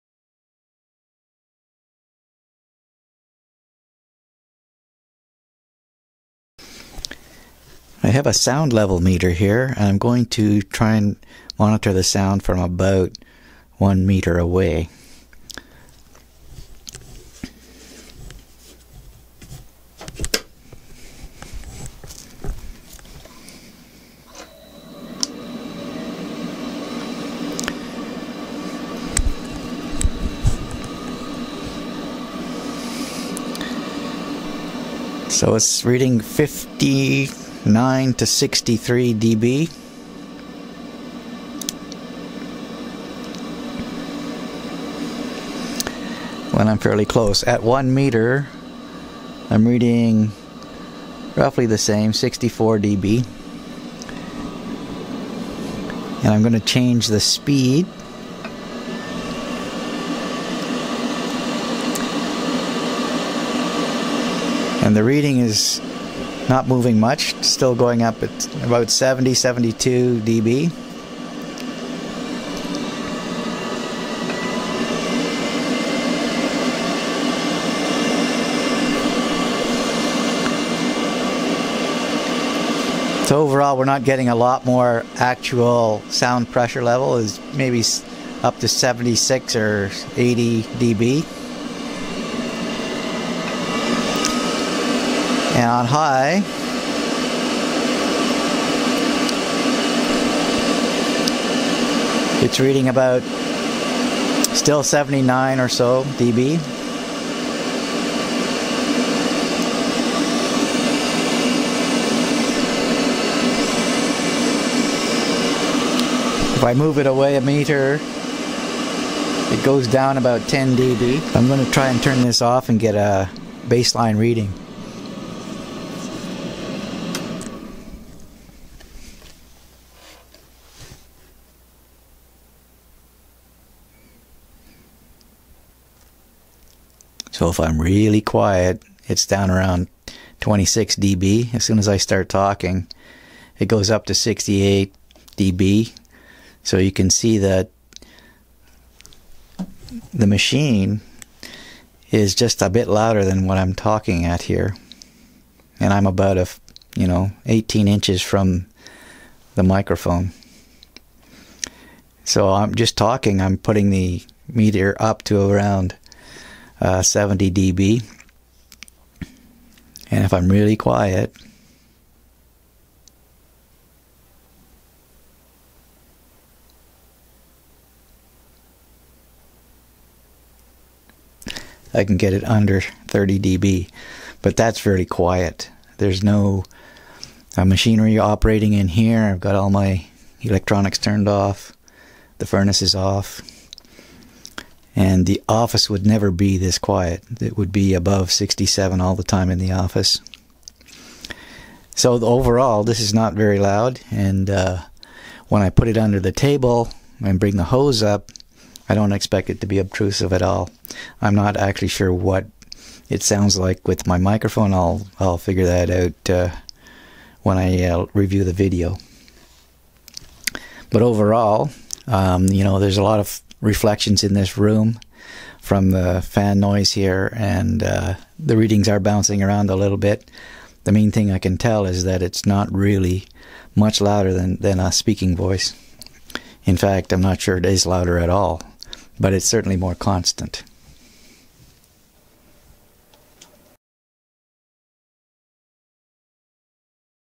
WellerSoundLevel1
Weller Zero Smog EL Kit 1 - Sound Level Measurements